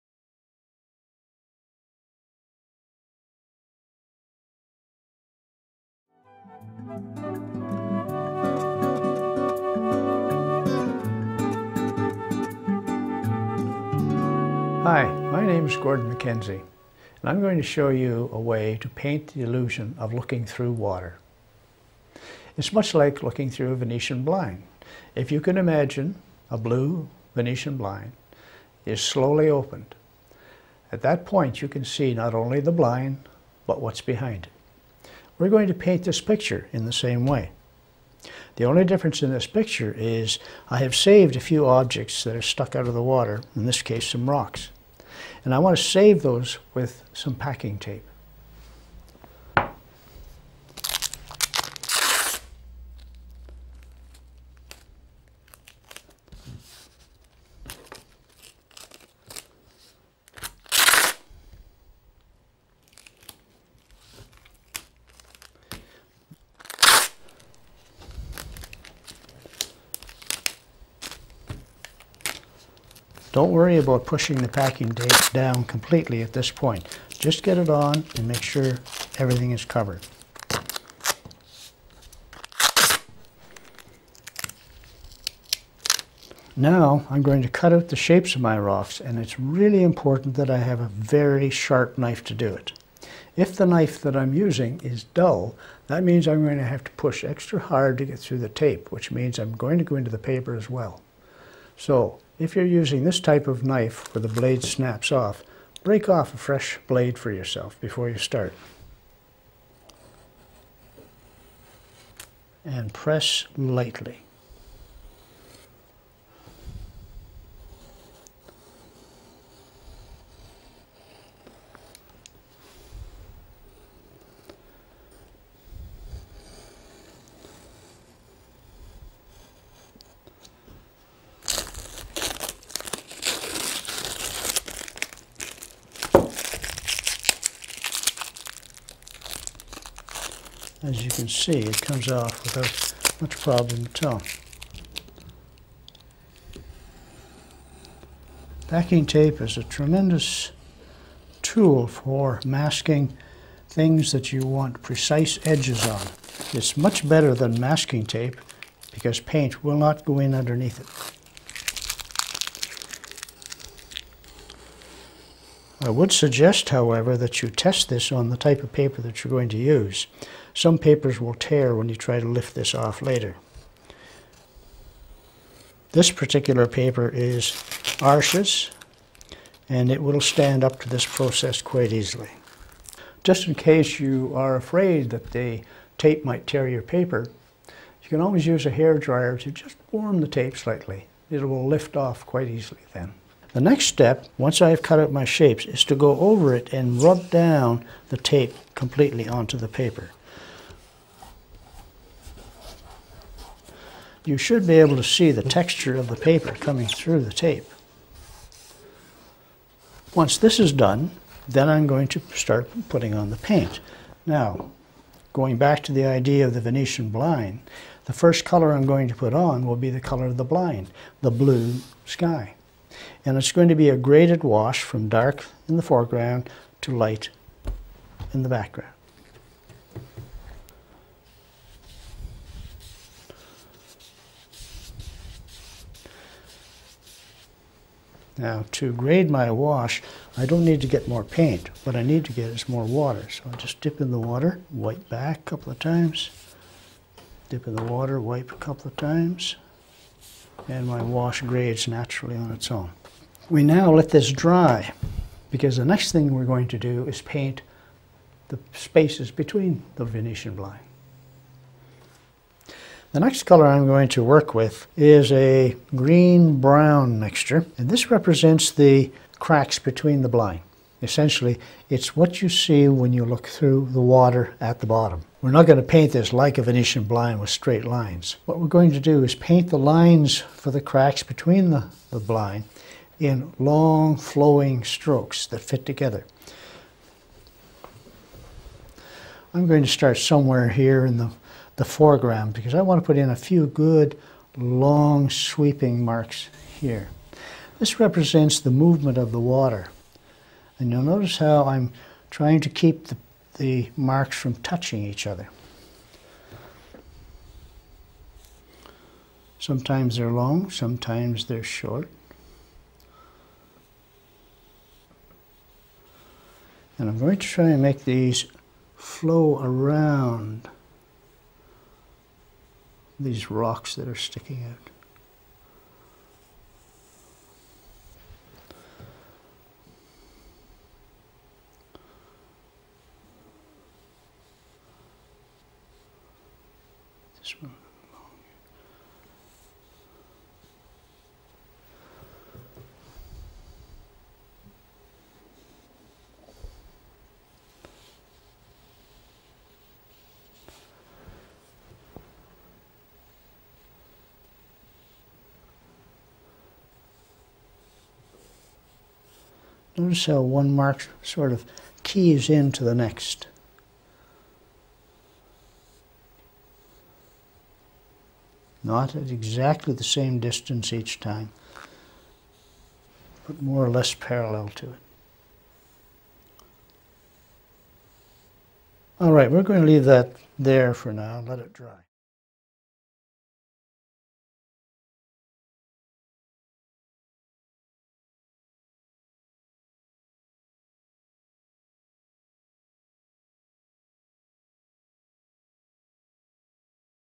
Preview | Improve Your Water Painting Techniques in Watercolor with Gordon Mackenzie
Learn how to paint water flowing around rocks using easy watercolor painting techniques in this excerpt from Gordon MacKenzie's video, Improve Your Water Painting Techniques in Watercolor, now playing

In his video, Gordon demonstrates three approaches to painting water, using watercolor painting tips to capture water flowing over rocks, adding rays of light and fish to a rock pond, and white-capped waves in a seascape.